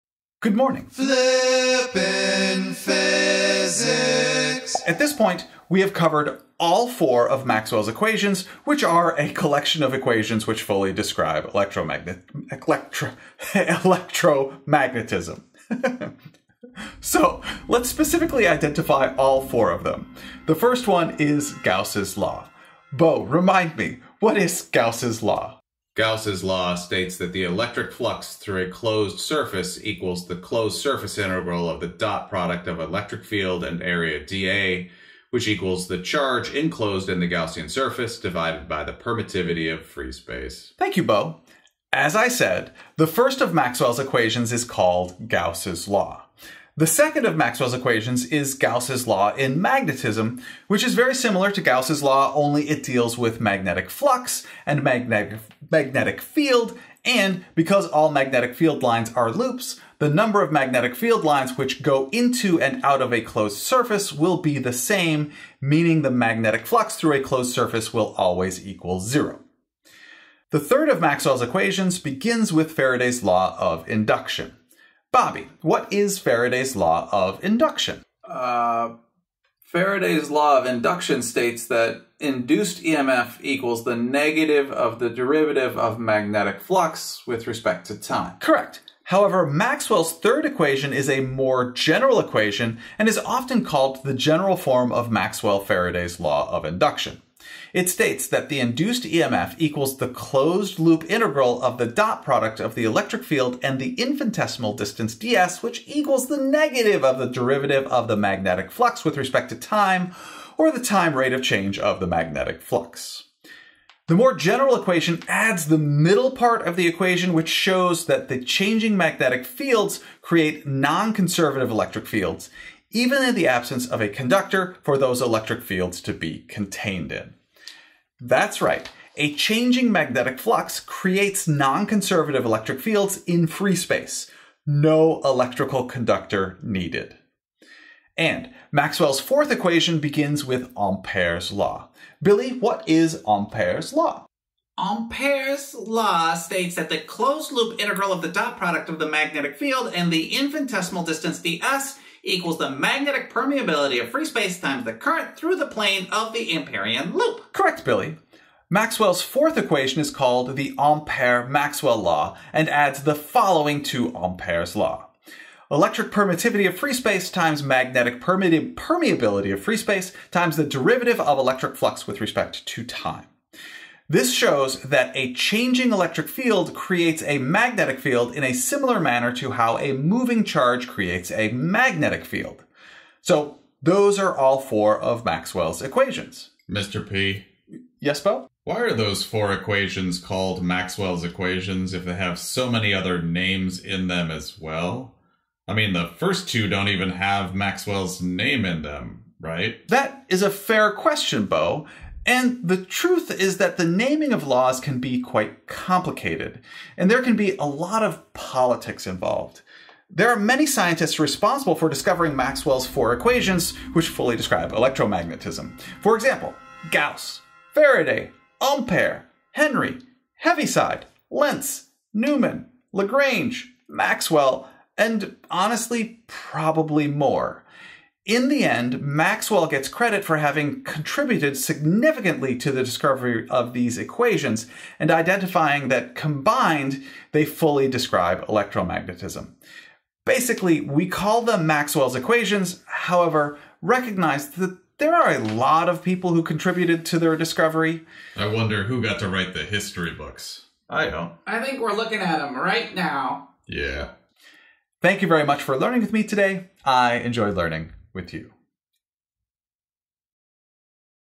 Maxwell's Equations
In this next episode, Mr. P guides us through the culmination of electromagnetic knowledge – Maxwell's equations. After a quick recap of Gauss's law and its magnetic counterpart, the class dives into Faraday's law of induction, unveiling its general form that reveals changing magnetic fields generate nonconservative electric fields. The grand finale, Ampère-Maxwell law, showcases the interplay of changing electric fields creating magnetic fields. Mr. P clarifies the complex nomenclature, acknowledging the multiple contributors to these fundamental equations, and explains why they're commonly attributed to Maxwell. Uncover the intricate web of electromagnetic principles with Maxwell's equations.

Thank you to Adithya Bhaskara, Julie Langenbruner, and Carl Hansen for being my Quality Control Team for this video.

Chapters:
0:00 Maxwell’s Equations
0:21 Gauss’s Law
0:48 Gauss’s Law for Magnetism
1:12 Maxwell-Faraday’s Law of Induction
2:20 Maxwell-Ampère’s Law
3:21 Many, Many Scientists!